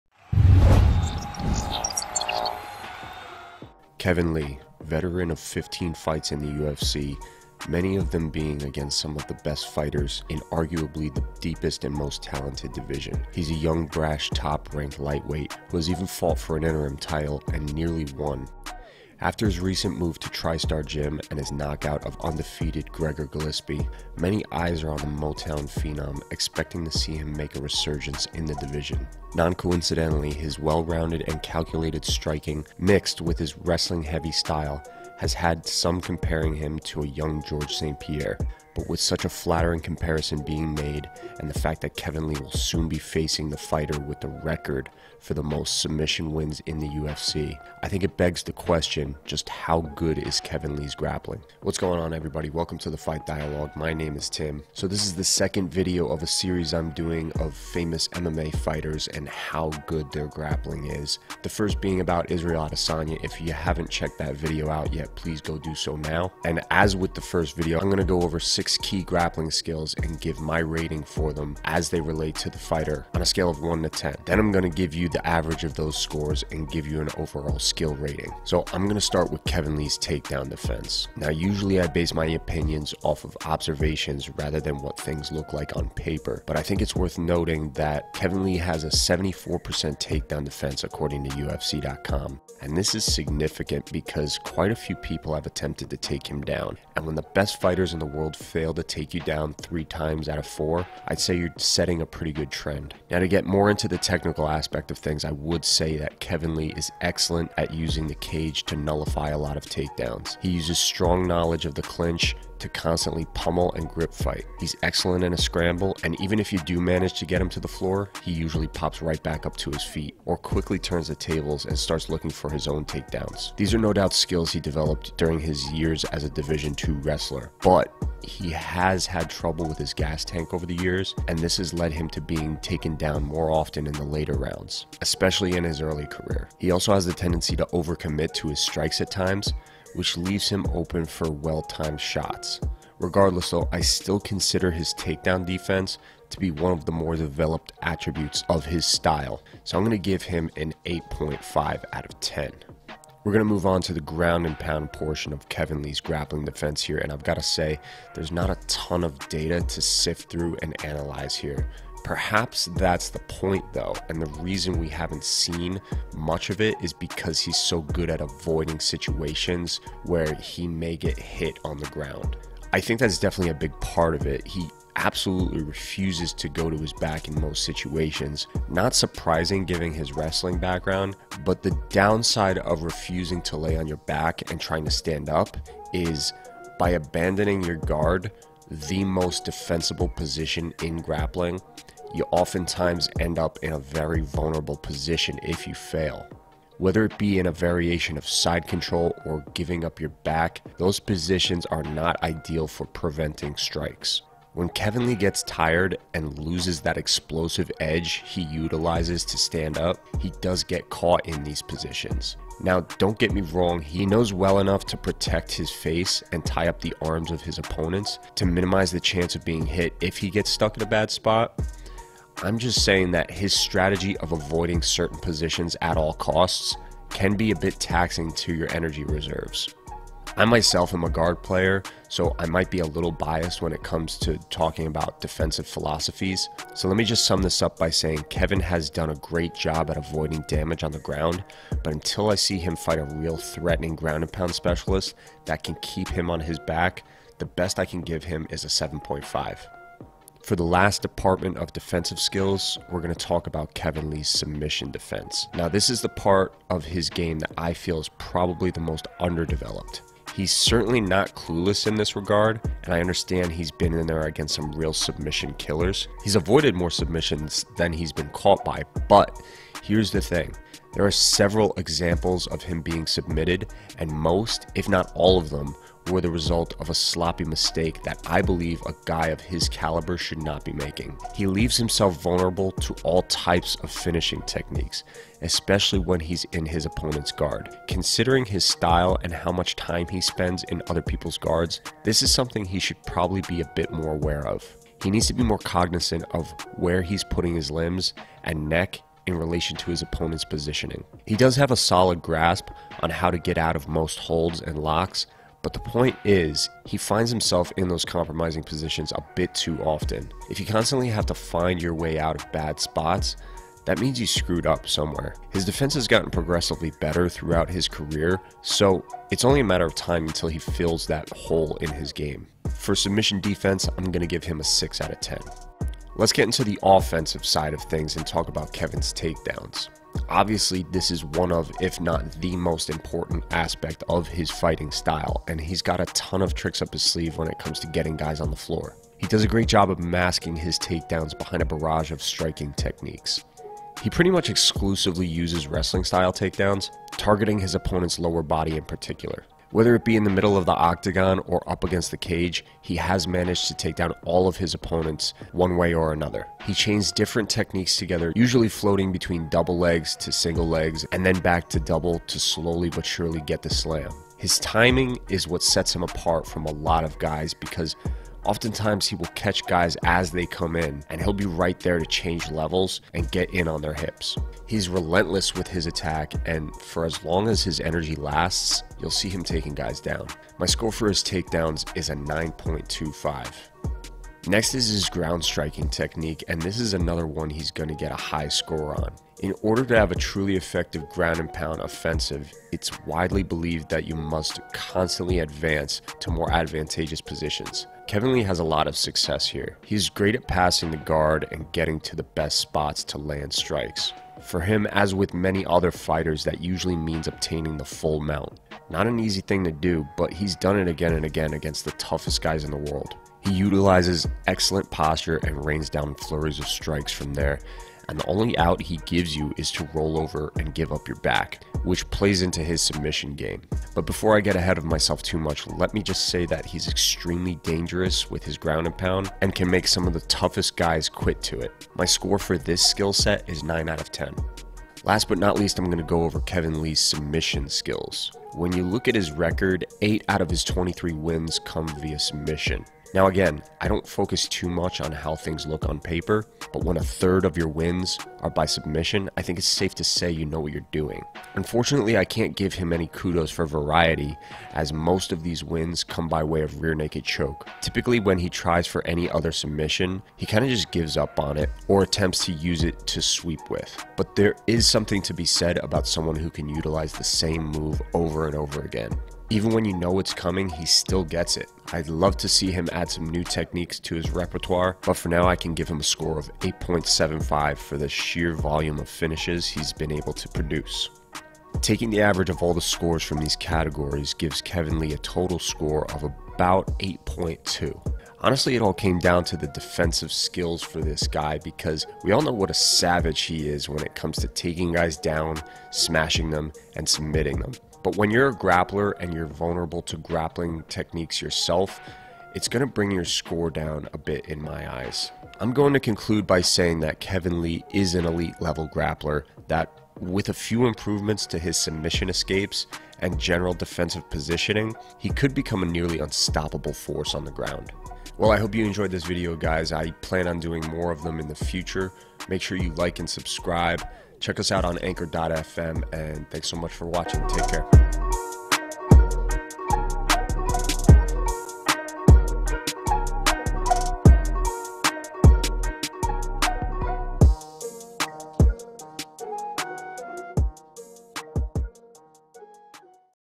How Good is Kevin Lee's Grappling?
The second episode in my series critiquing the grappling abilities of the worlds best MMA fighters, Kevin Lee is a fighter i greatly respect but at times I've noticed his performances can be a bit inconsistent. I thought it would be a great opportunity to take a look at his ground skill set considering he will be facing one of the most feared ground specialists in Charles Oliveira.

Help us grow!! Follow the link below for great SANABUL RASH GUARDS!

Support our podcast on Anchor!